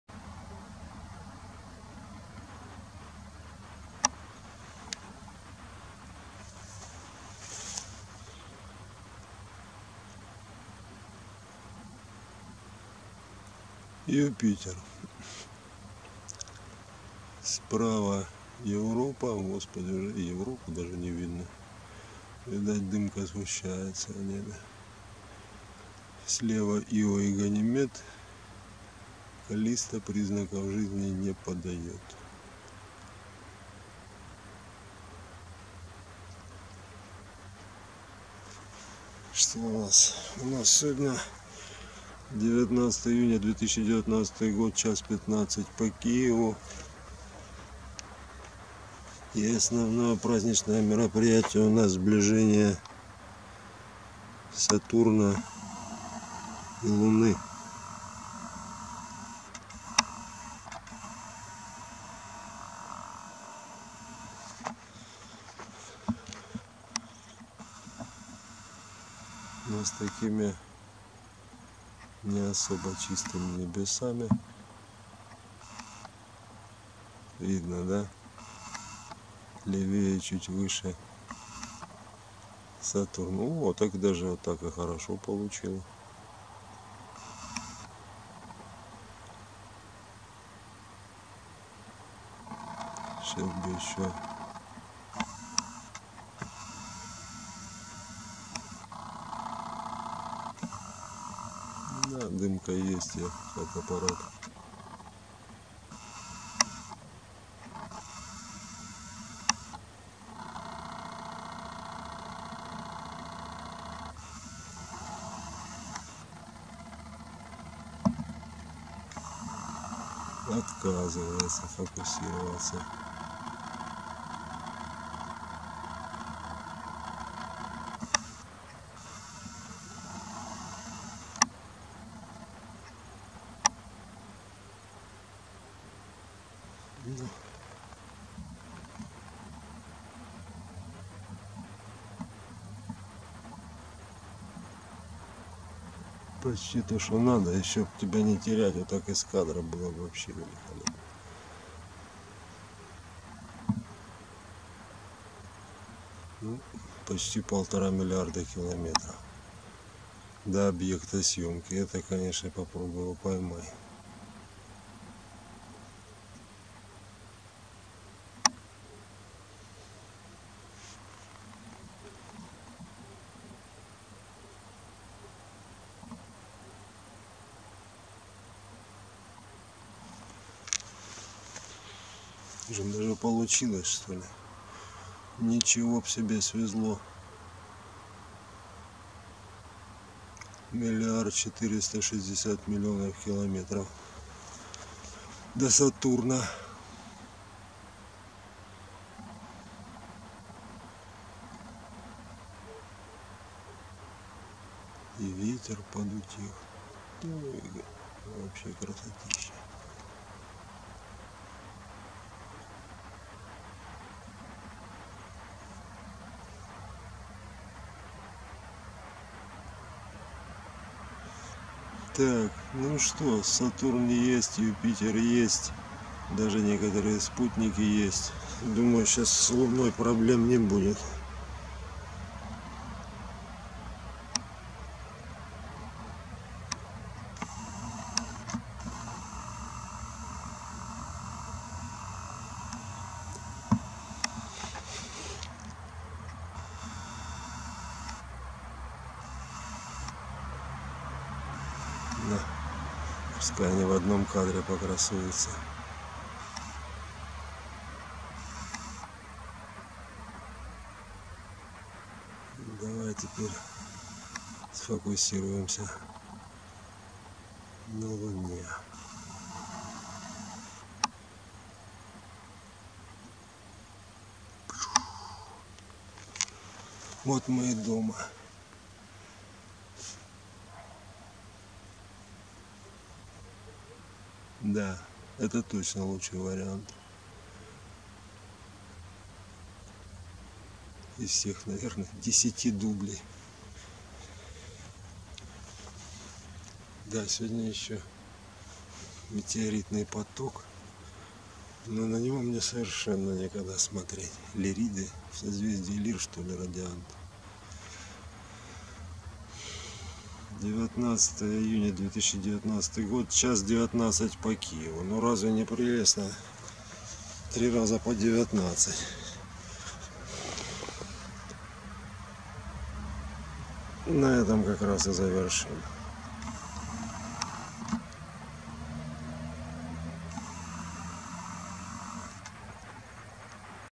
Сближение Сатурна и Луны 19 июня 2019 Юпитер и спутники
Луна и Сатурн на ближайшем расстоянии на небосводе 19 июня 2019 года.
Юпитер с галилеевыми спутниками: Ио, Европа, Ганимед, Калисто.